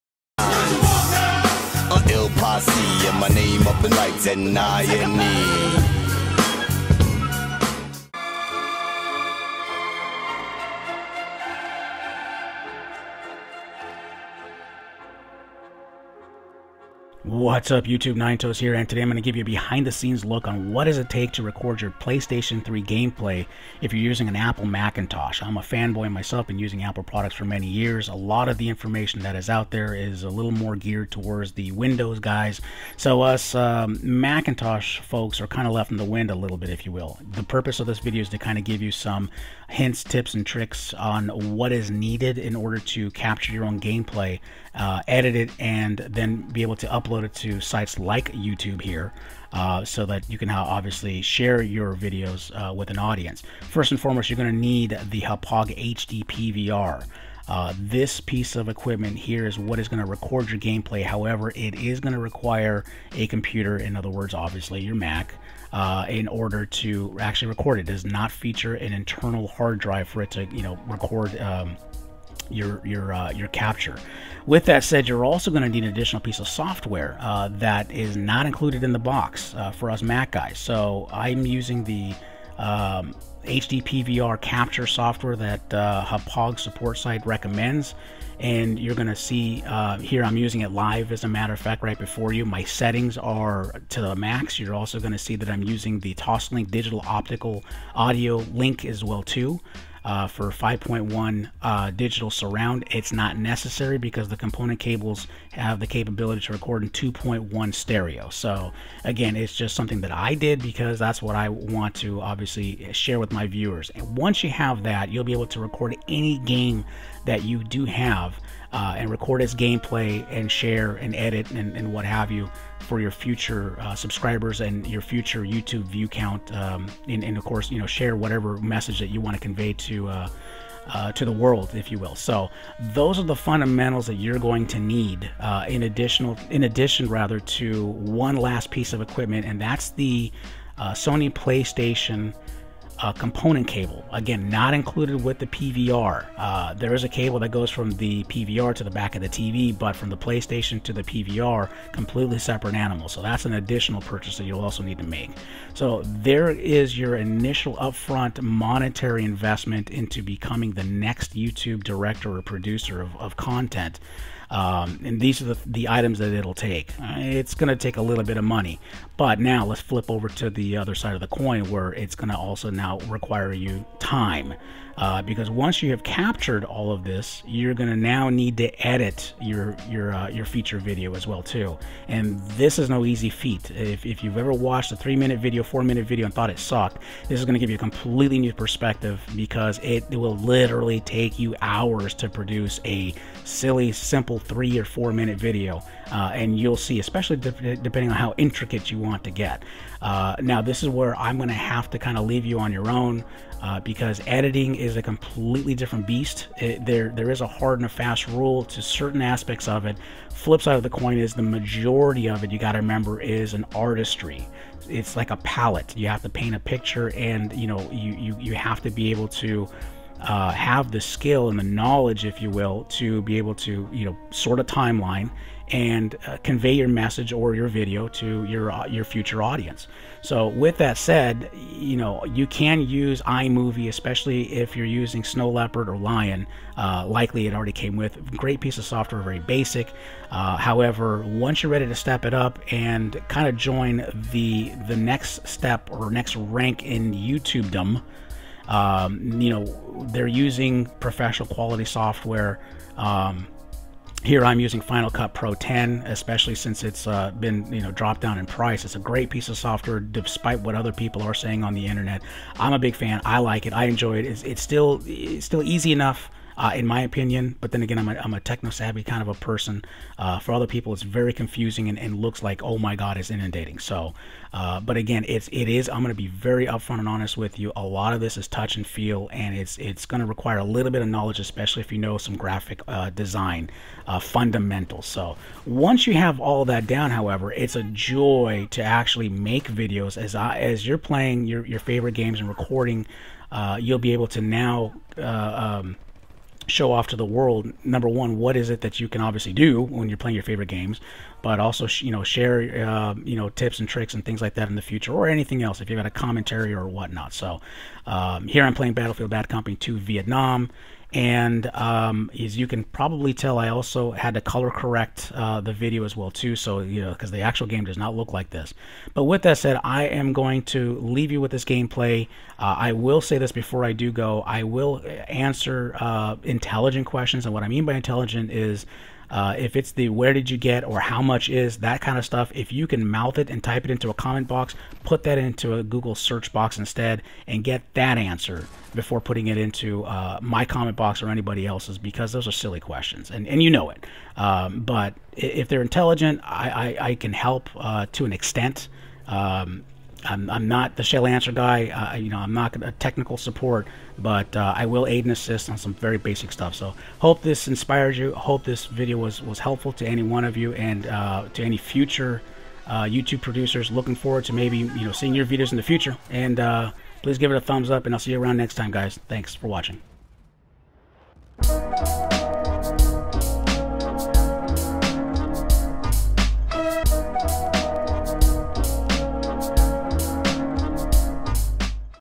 How To: Record Your PS3/Xbox 360 Gameplay on a Mac Tutorial
I walk through what you'll need in order to do so however just like anything Apple, quality does come with a price. I'll show you my setup and what I use in order to make professional grade videos.

PLEASE NOTE THAT WHILE I'M WILLING TO ANSWER SELECT QUESTIONS LEFT IN THE COMMENTS (please, no private messages), PLEASE GOOGLE SEARCH FIRST.

Hauppauge HD-PVR 1212
Elgato Game Capture HD
Sony PS3 component cable
Microsoft Xbox 360 component cable